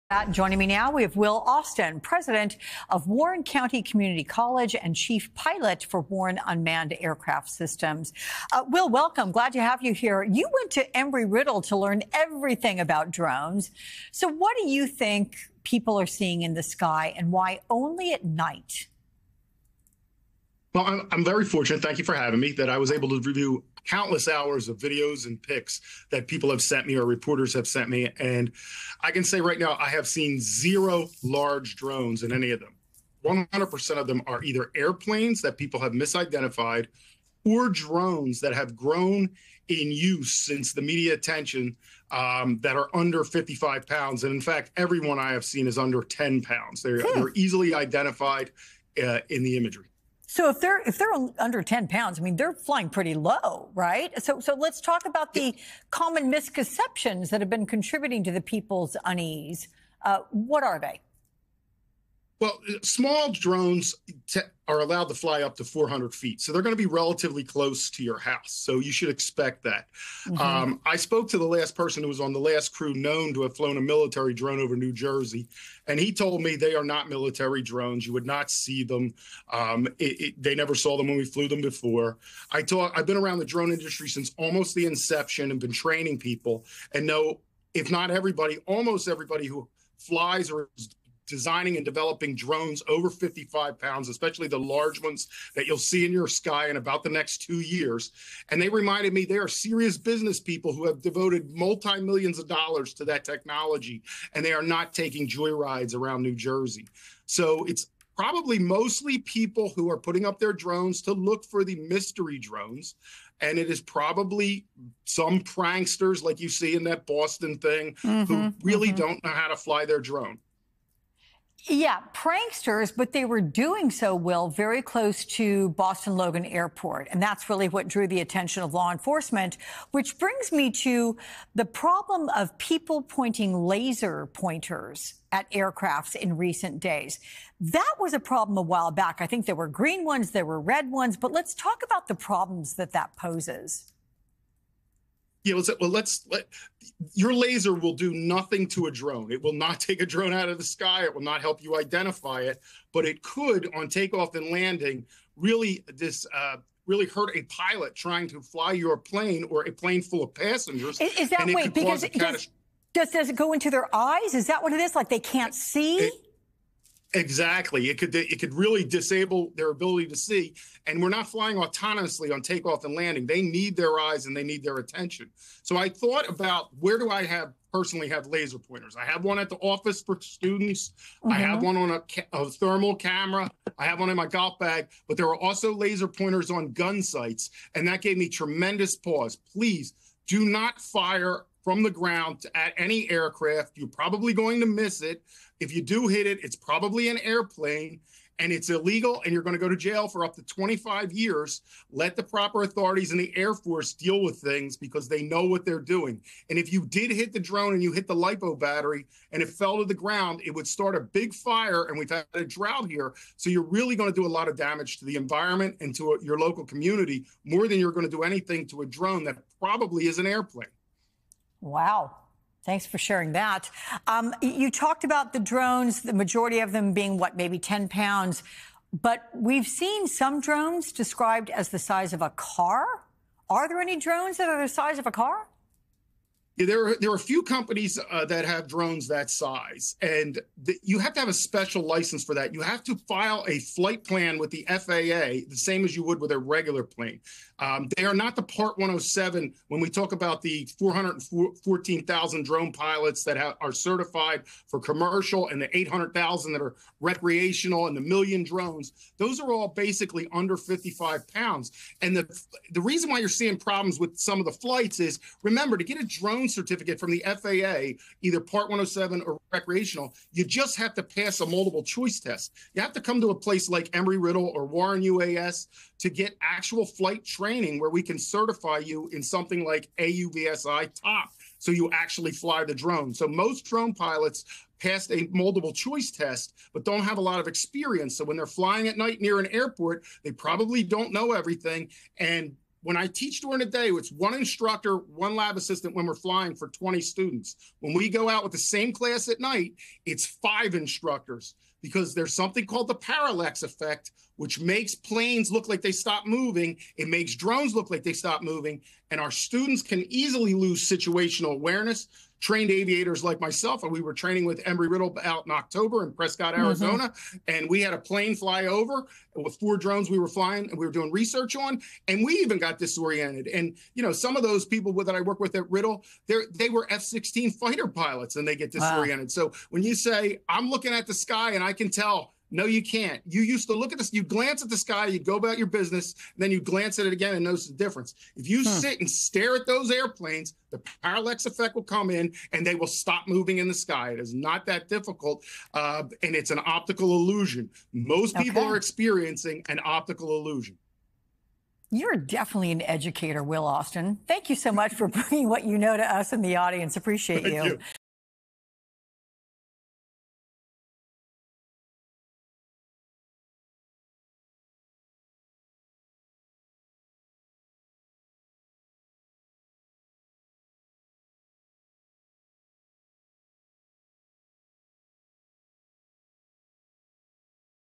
Drone expert debunks mystery sightings and explains how lasers can be deadly to passing pilots
Unidentified objects, thought to be drones, have been spotted around military bases and secure facilities around the country for the last week. Some Americans have taken it upon themselves to try and identify them as officials downplay their threat. Warren Unmanned Aircraft Systems Chief Pilot and Warren County Community College President, Will Austin, discusses what they could be and more.